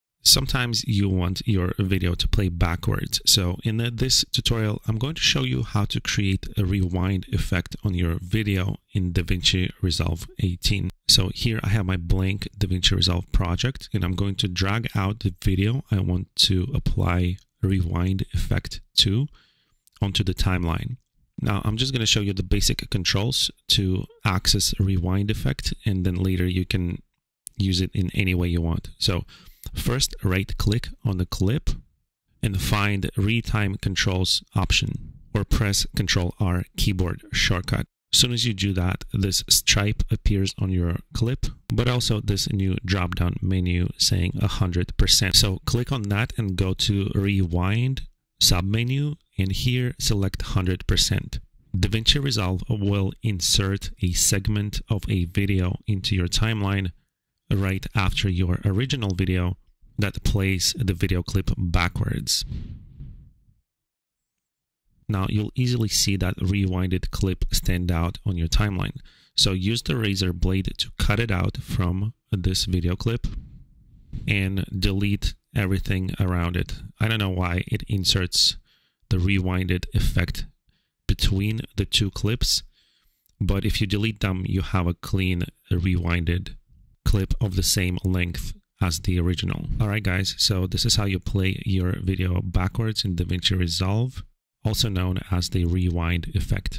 DaVinci Resolve Rewind Effect (play video backwards)
davinciresolve how to replay your video backwards in your video clip in DaVinci Resolve 18 / play video backwards /

Video Chapters:
00:00 Rewind Intro
00:26 Retime Controls
00:39 Rewind 100%
01:03 Crop

How To Change Resolution In Davinci Resolve 18 To Vertical 9:16